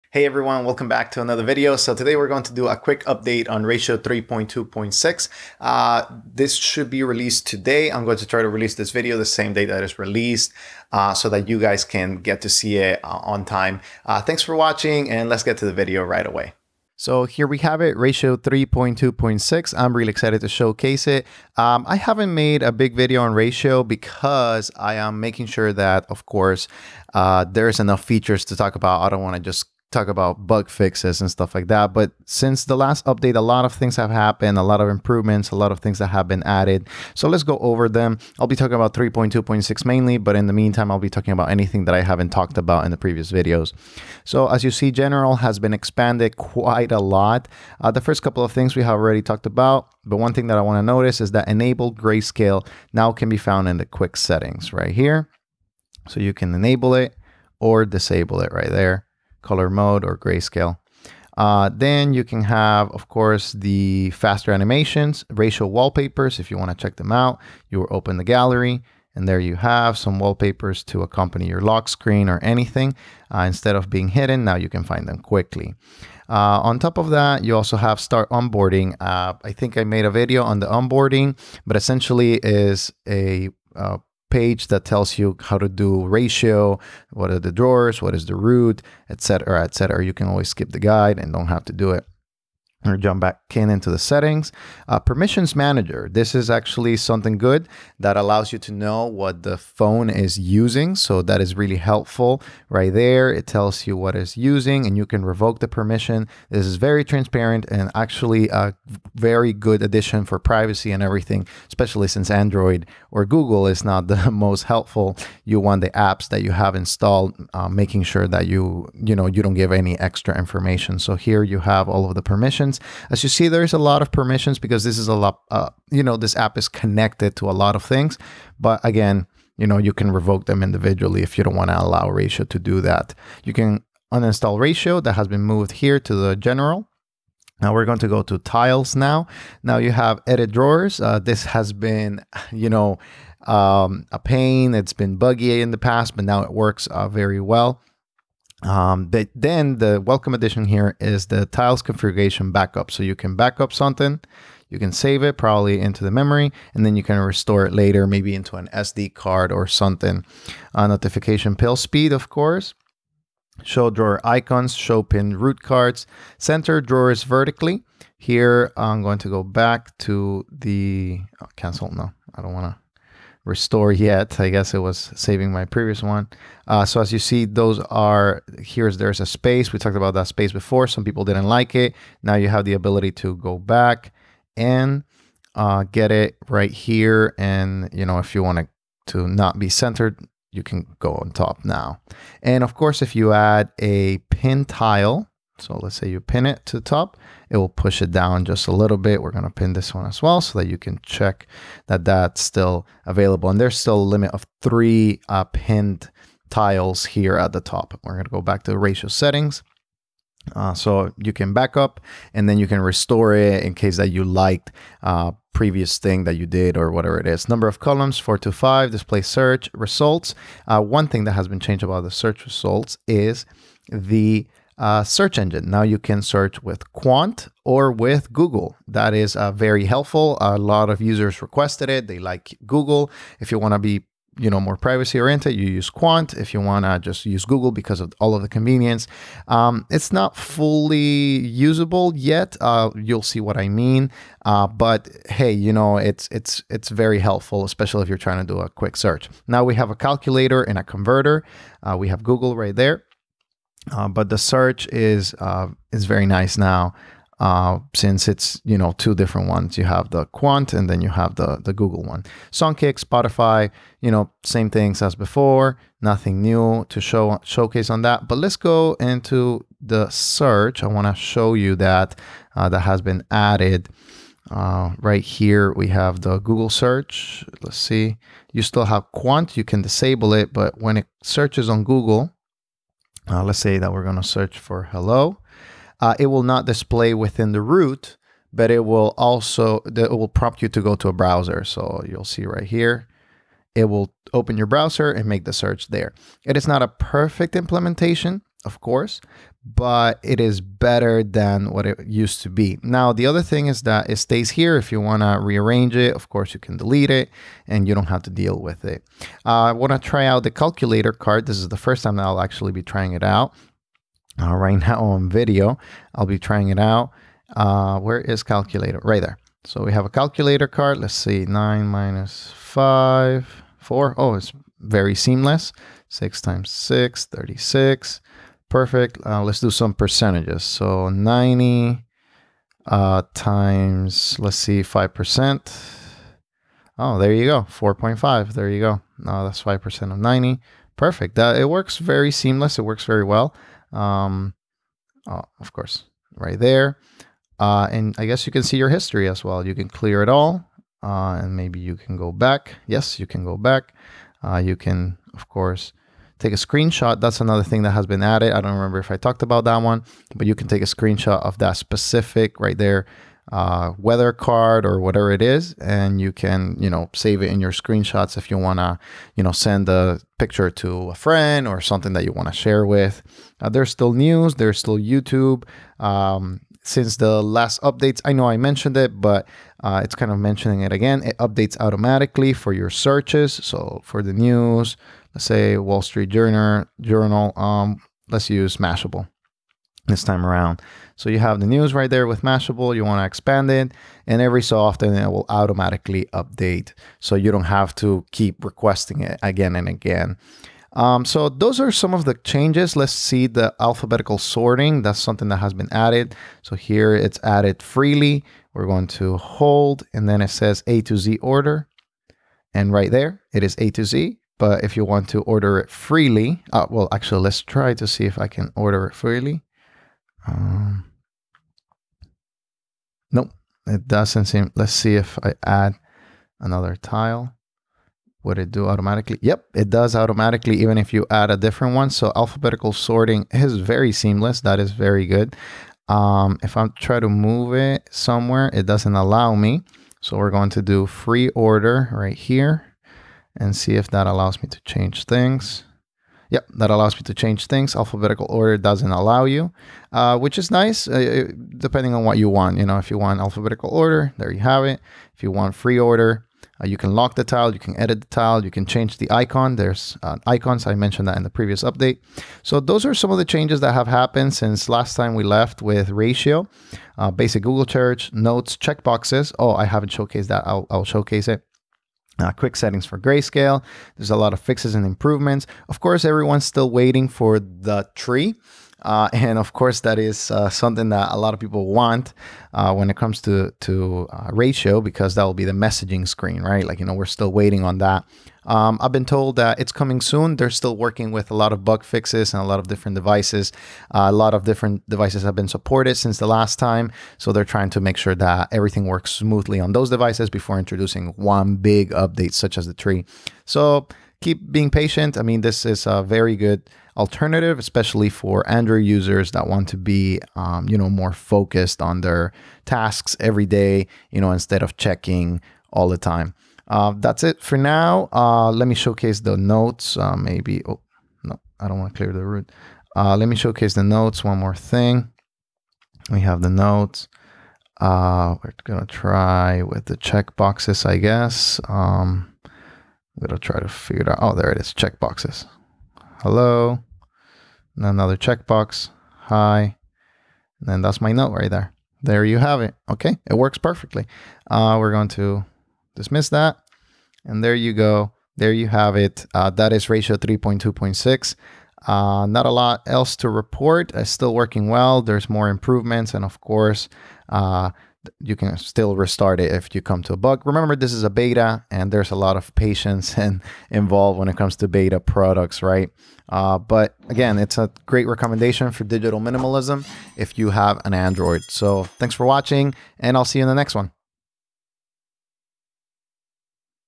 Ratio Launcher 3.2.6 || What's New!
Today we take a look into Ratio Launcher version 3.2.6. Some nice new additions since the last time I gave an update on the development! The Tree is coming, but not yet. Patience is a virtue so I hear :)

Chapters ⏲⏲
0:00:00 Intro
0:00:30 Ratio Launcher 3.2.6
0:01:07 Wallpapers
0:01:21 Onboarding Experience
0:01:38 Permissions Manager
0:02:23 Tiles Features
0:02:50 Centered Drawers
0:03:50 Root Features
0:04:46 Google Search
0:05:20 Calculator
0:06:45 Alphabetical Sorting
0:08:00 Extras and Updates about the Tree
0:08:50 Notes and Checkboxes

Habit Calendar
Habit Journal

Books 📚📚
Atomic Habits
The Power of Habit
The Shallows
7 Habits of Highly Effective People